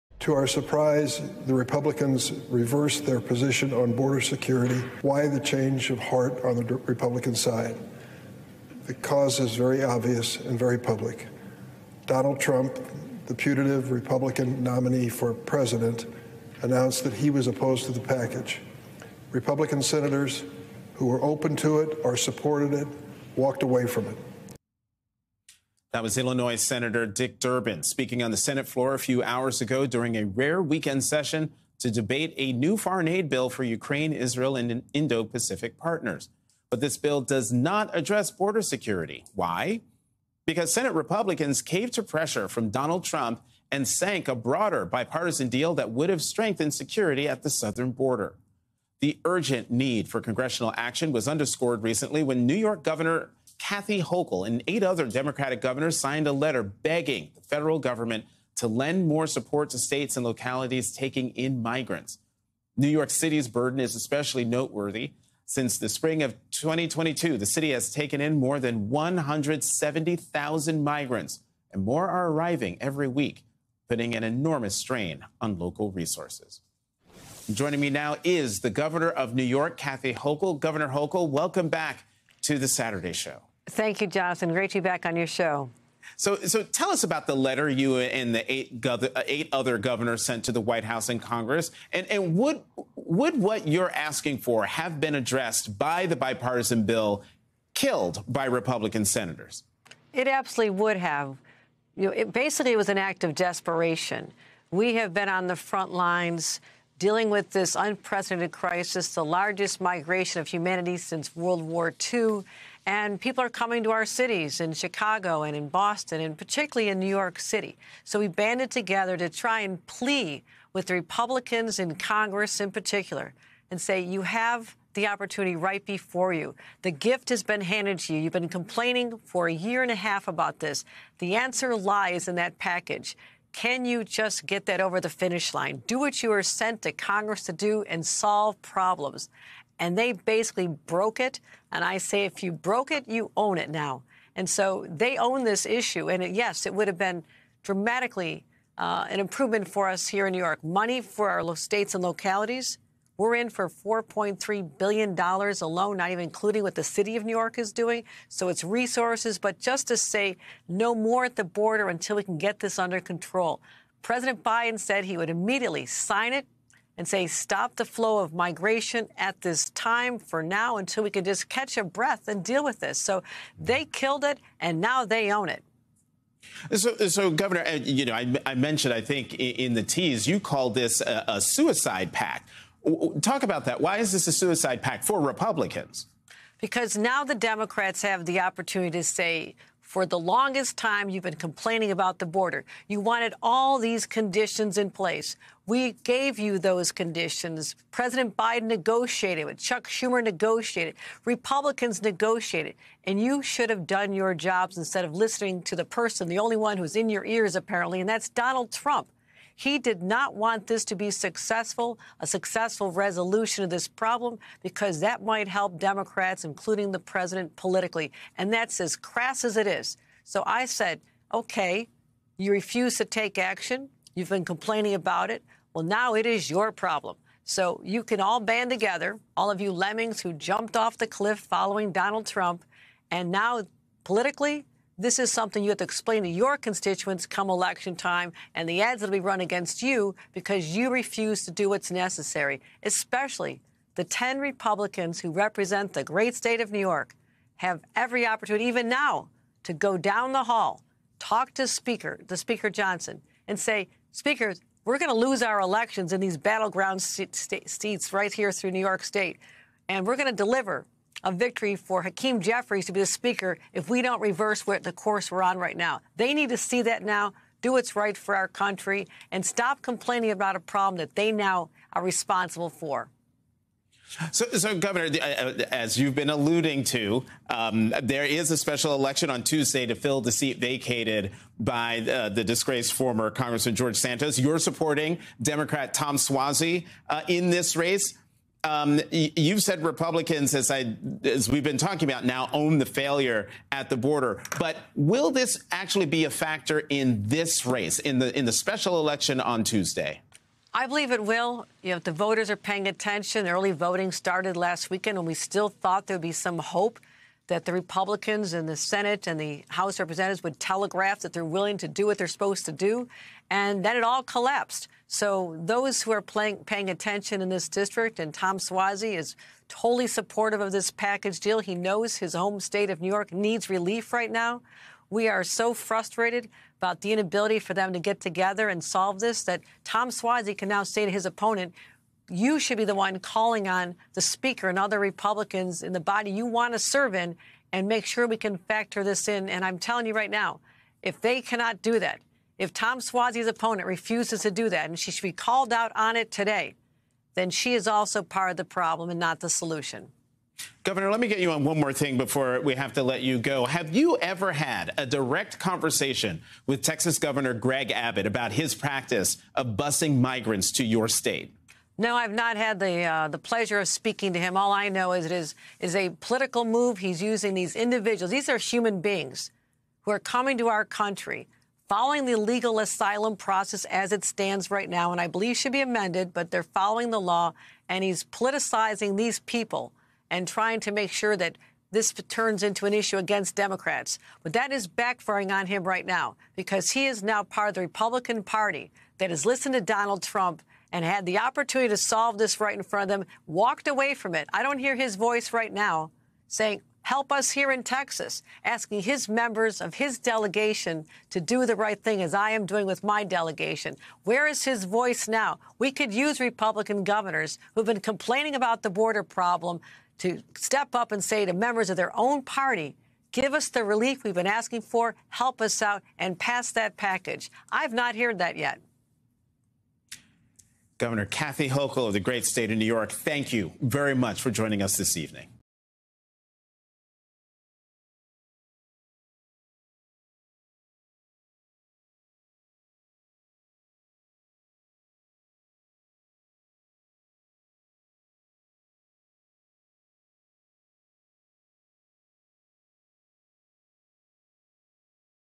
NY Governor Kathy Hochul reacts to congressional inaction on the southern border
As Republican lawmakers continue blocking every attempt to help the situation at the southern border, states and localities taking in the migrants are still struggling. Over 170,000 migrants have arrived in New York City since Spring, 2022, putting enormous strain on the city's resources. New York Governor Kathy Hochul joined MSNBC's Jonathan Capehart to discuss this difficult dynamic.